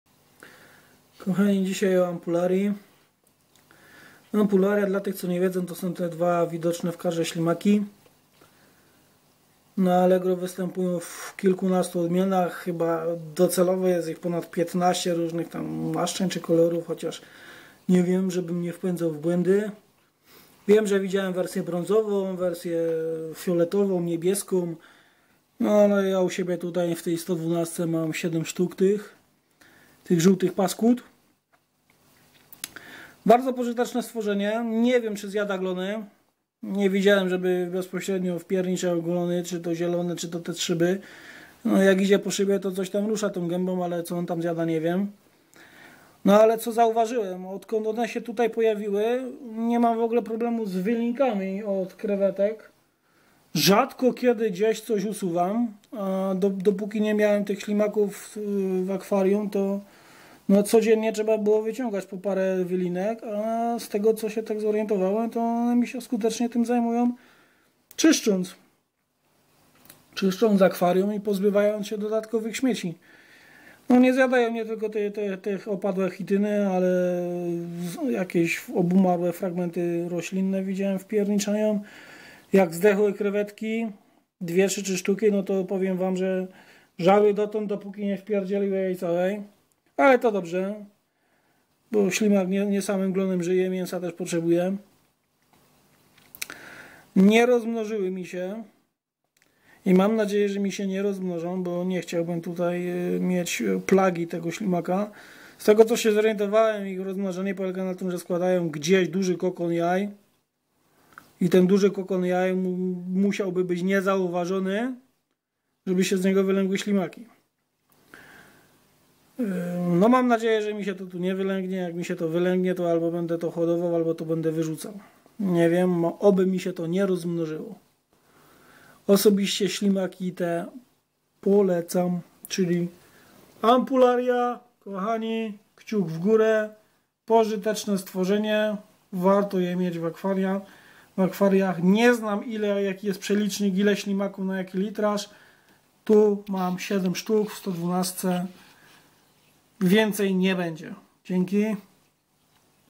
Ampularia ślimak czyszczący
Pożyteczny ślimak czyszczący. Nie stanowi plagi w akwarium. Zauważyłem ze zjadają nawet pancerzyki z krewetek, dzięki temu mam mniej prac sprzątających.
Skutecznie sprzątają pozostałości po pokarmie.
Osobiście mam narazie odmianę żółtą.